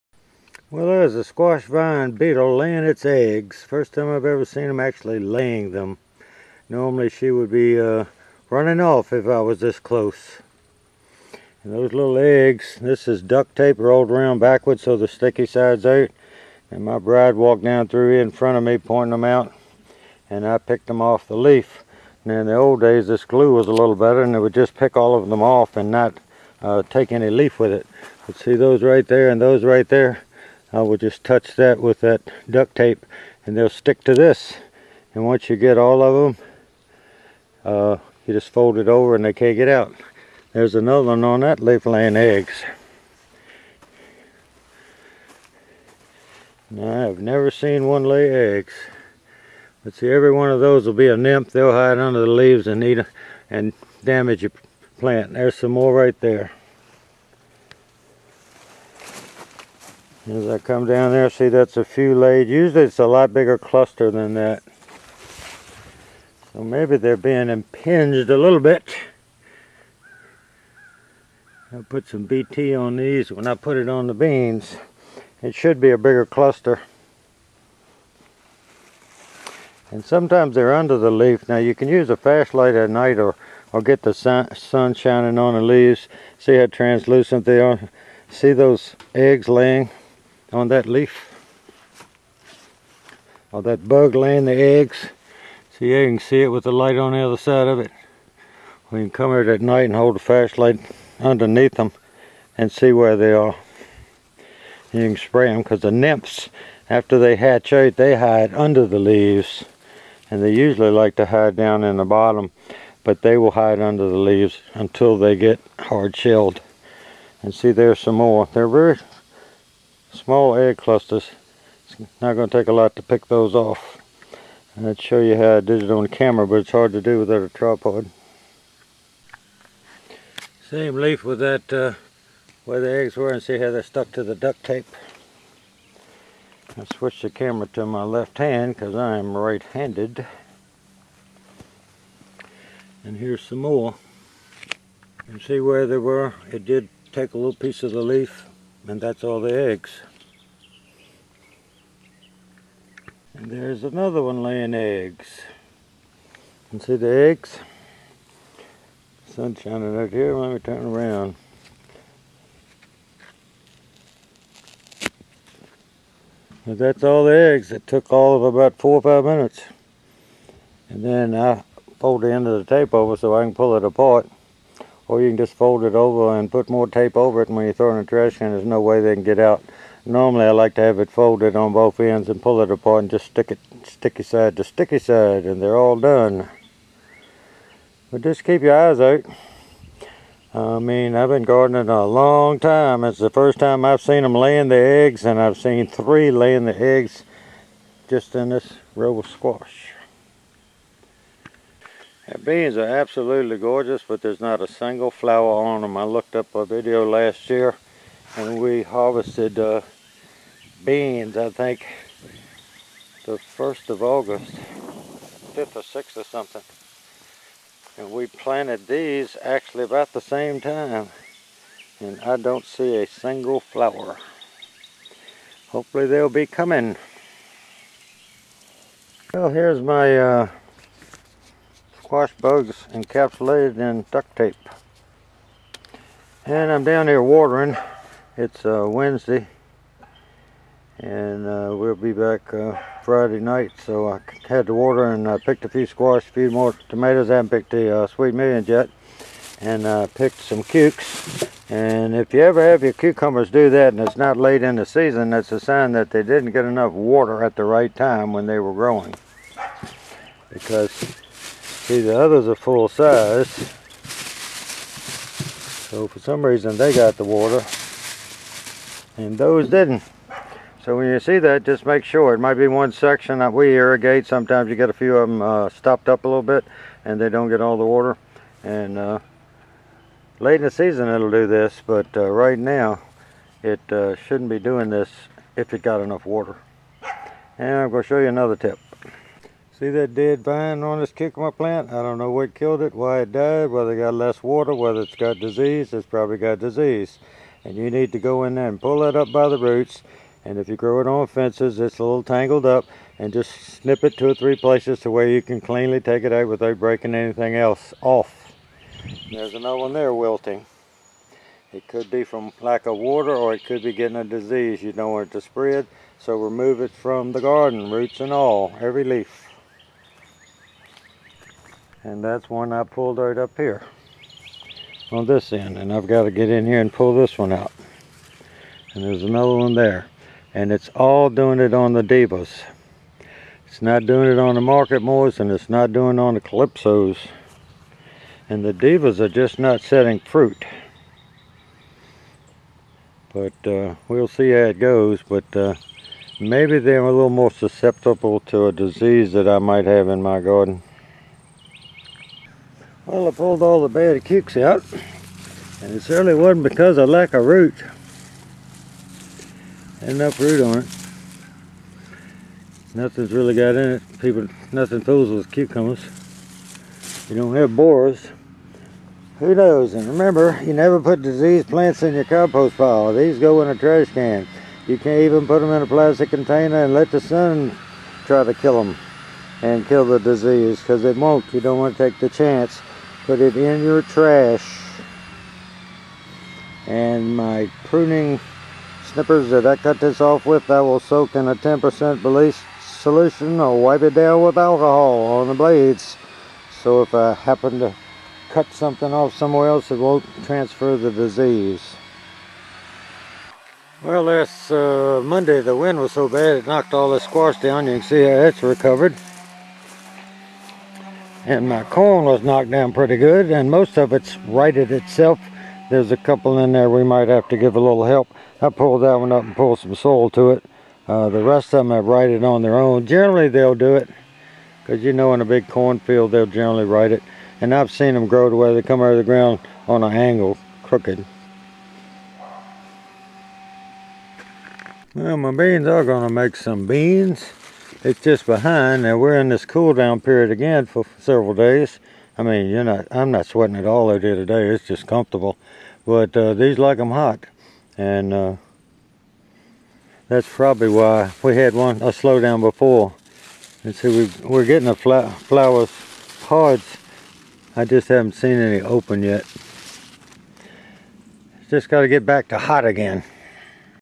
TIPS ON REMOVING SQUASH BUGS AND CUKES
Cucumbers can also mature as round balls with wither ends if the soil lacks some mineral that the plant needs. That is why it happens more toward the end of the growing season rather than early on when the fertilizer with trace minerals are still active in the soil. If you have just a few like these in the video the fertilizer might not have been spreaded out evenly in all of the soil. Incomplete fertilization of the flower can also cause this condition. Insects can also cause a plant to wilt. They are still edible.